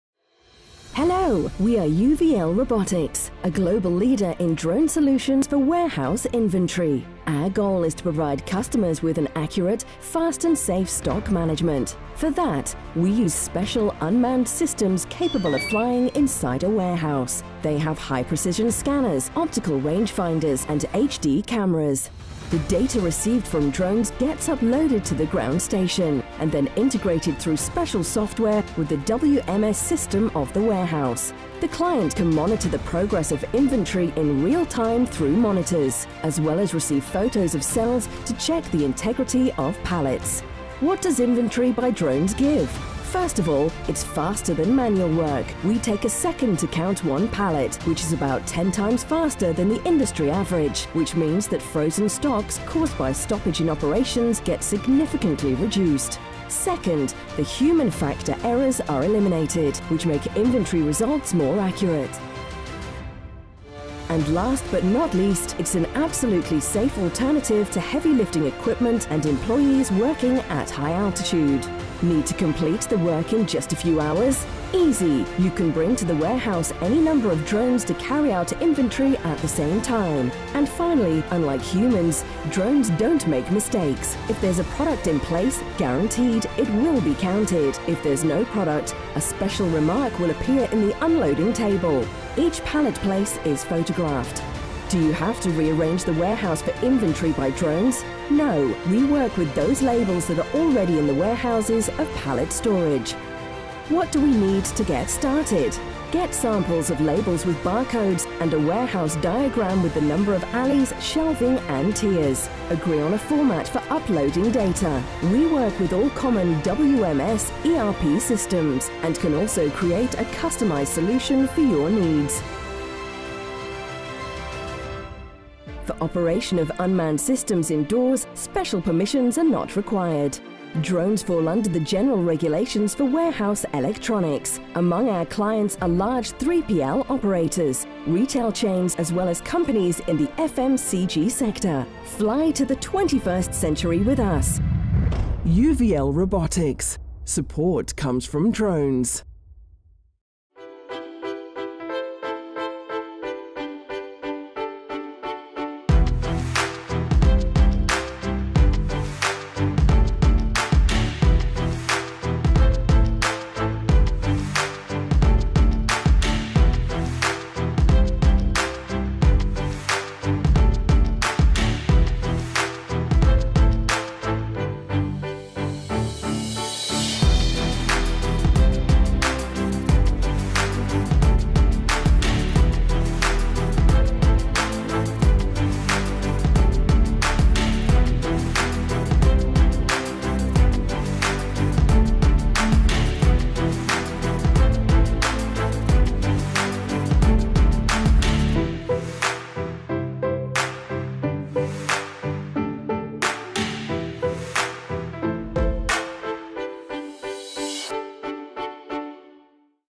UVL Robotics I official teaser EN
Introducing UVL warehouse drones for inventory counts improvement.

Autonomous Inventory counts solution (1 hour for 10k pallets) for indoor warehouse management with WMS/ERP integration.